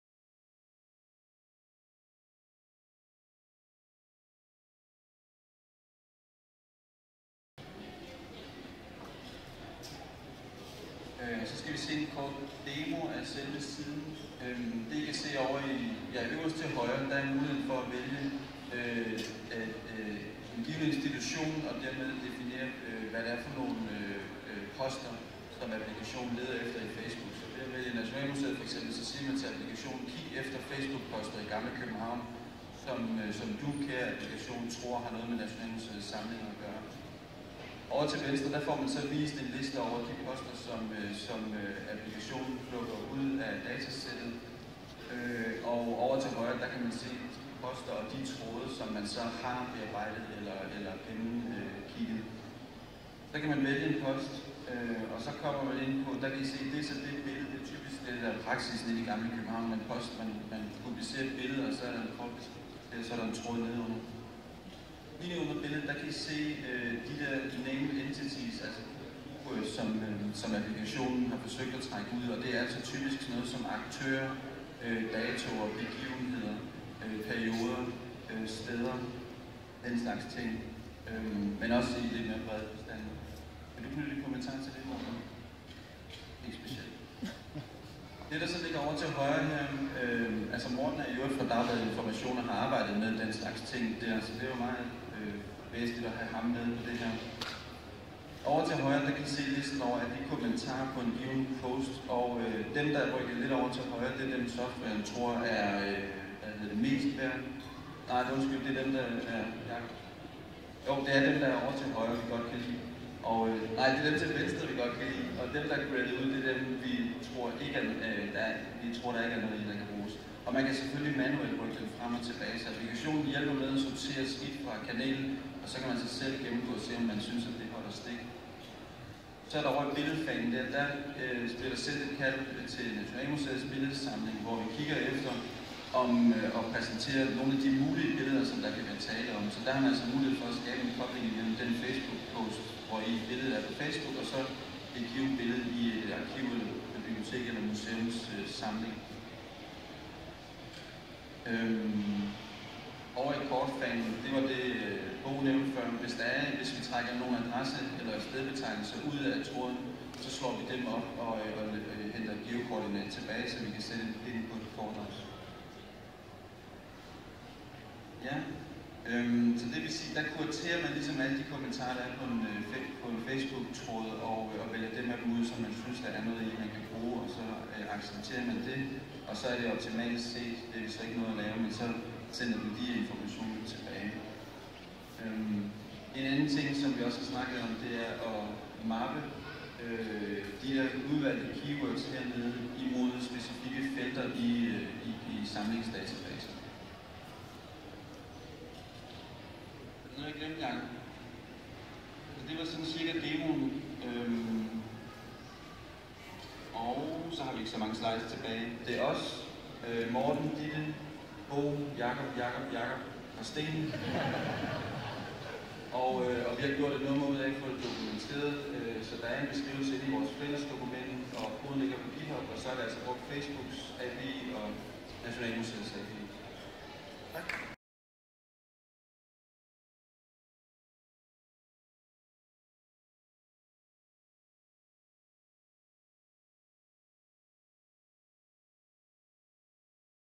HACK4DK 2014HeritageEvacuator part 2
The group working with the project Heritage Evacuator presents their results at HACK4DK 2014